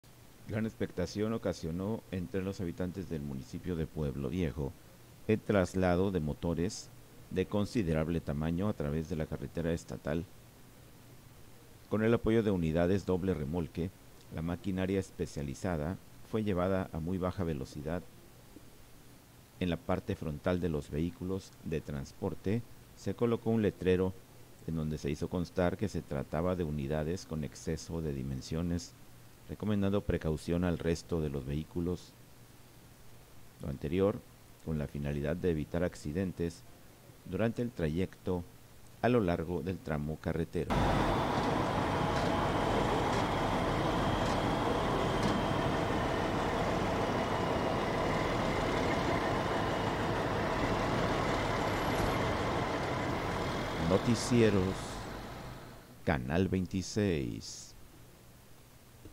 Genera Expectación en Tramo Carretero Traslado de Motores Industriales en Remolques
Gran expectación ocasionó entre los habitantes de Pueblo Viejo, el traslado de motores de considerable tamaño, a través de la carretera estatal.

Con el apoyo de unidades doble remolque, la maquinaria especializada fue llevada a baja velocidad.

En la parte frontal de los vehículos de transporte fue colocado un letrero en donde se hizo constar que se trataba de unidades con exceso de dimensiones.

Lo anterior, con la finalidad de evitar accidentes durante el trayecto a lo largo del tramo carretero.